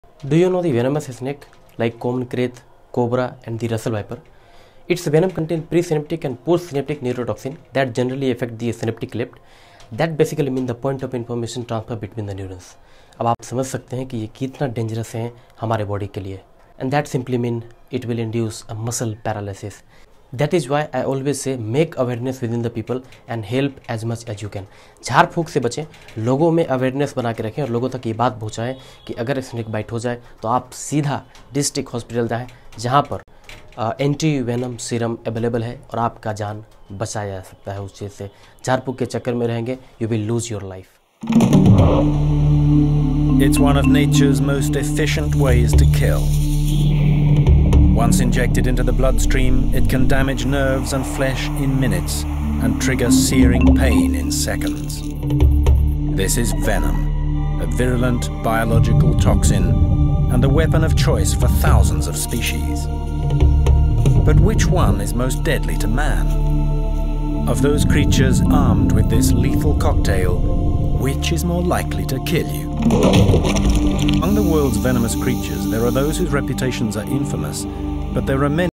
साँपों के काटने के बाद क्या होता है ॥ भारत का सबसे ज़हरीला साँप॥
Snake bite is an important occupational and rural hazard because India has always been a land of Exotic snakes. In Maharashtra, common poisonous snakes are Cobra, Russell's Viper, Saw Scaled Viper, and Krait. It is a fact that inspite of heavy morbidity and mortality, very little attention is paid by the clinicians to this occupational hazard.